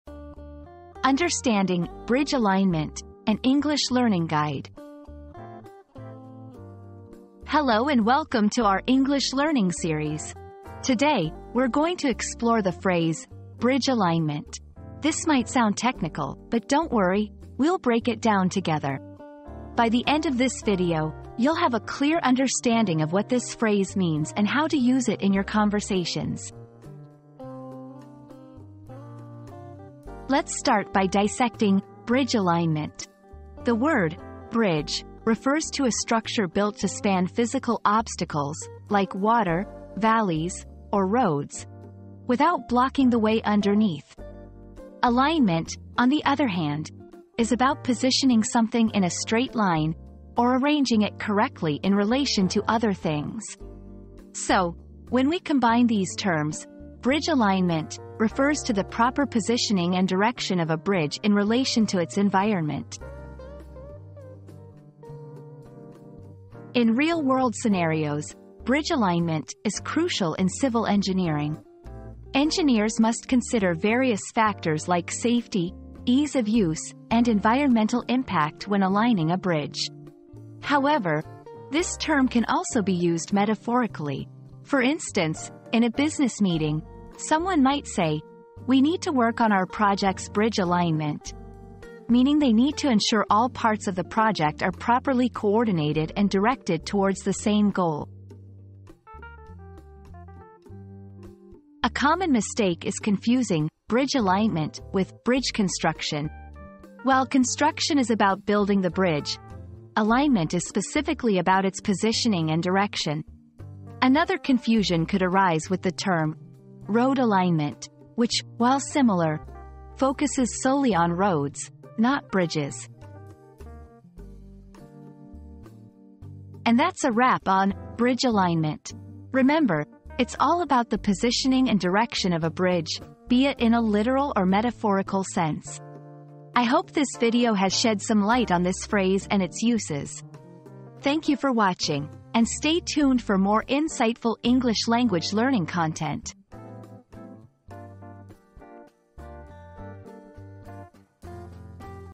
Understanding "Bridge Alignment" - An English Learning Guide
Unlocking Bridge Alignment: An English Learning Guide • Master the art of Bridge Alignment in English with this captivating learning guide. Discover the secrets to fluently connecting your thoughts and ideas, enhancing your communication skills to new heights.

00:00 • Introduction - Understanding "Bridge Alignment" - An English Learning Guide
00:29 • Breaking Down the Phrase
01:06 • Practical Applications
01:41 • Common Misunderstandings